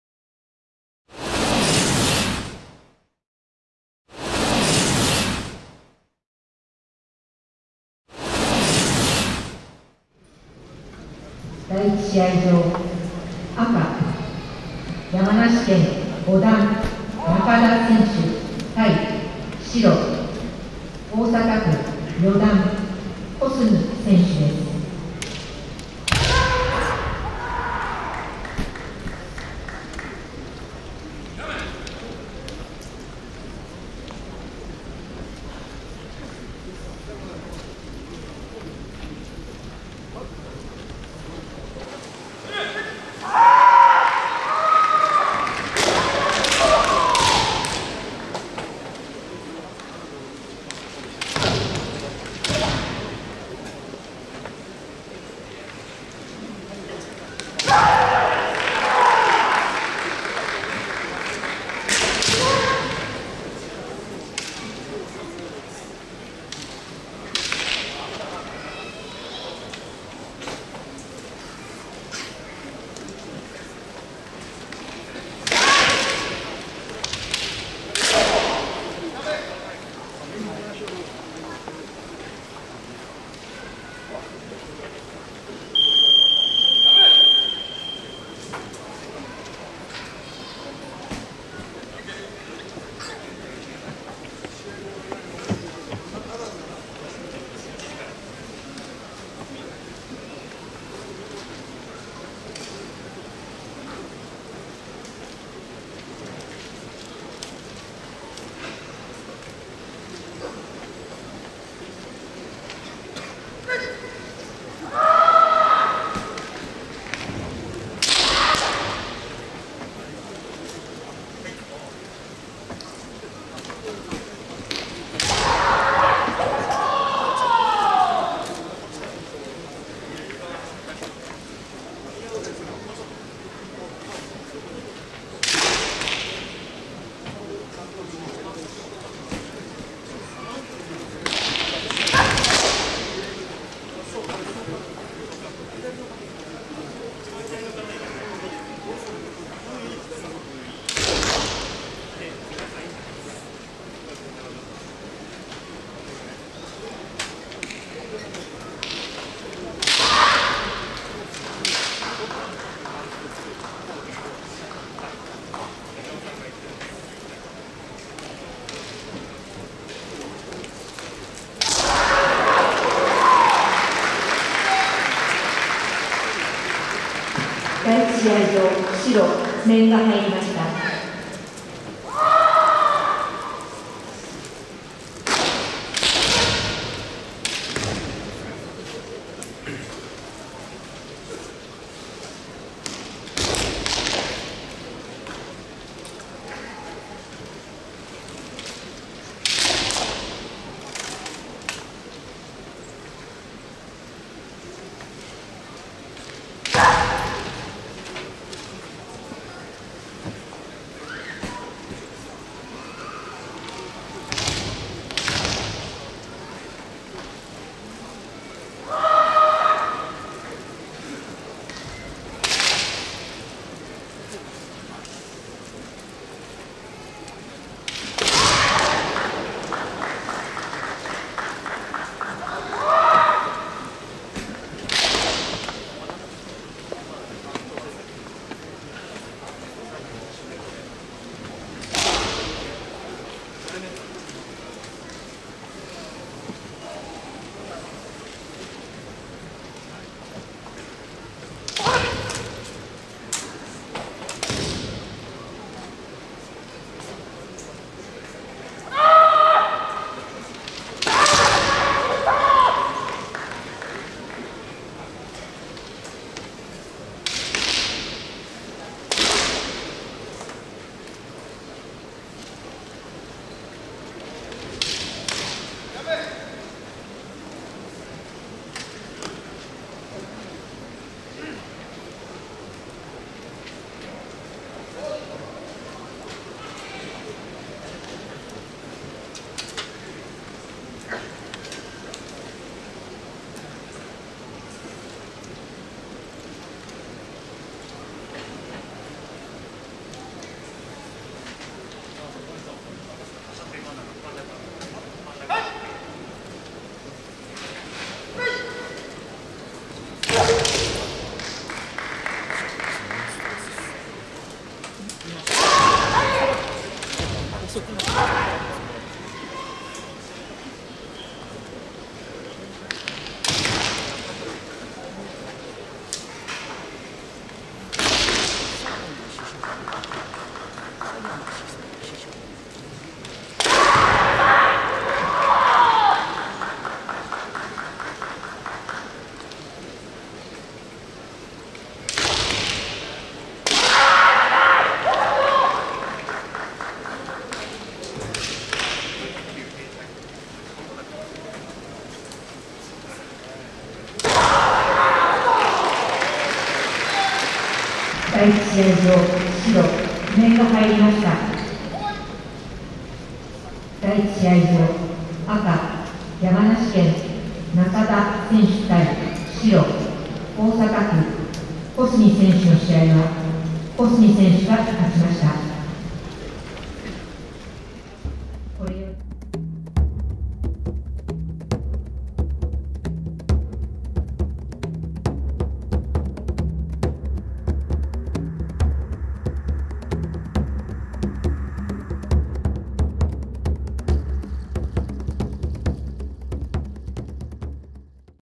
中田 圭紀 -メメ 小角 朋樹_第70回全日本剣道選手権大会 一回戦 16試合
第70回全日本剣道選手権大会 一回戦
令和 4年11月 3日(木)・日本武道館
70th All Japan KENDO Championship First round
Nov  3. 2022 (Thu) Nippon Budokan

[赤](山梨)中田 圭紀 -メメ 小角 朋樹(大阪)[白]
[Red](Yamanashi)Yoshiki NAKADA -MM Tomoki KOSUMI(Osaka)[White]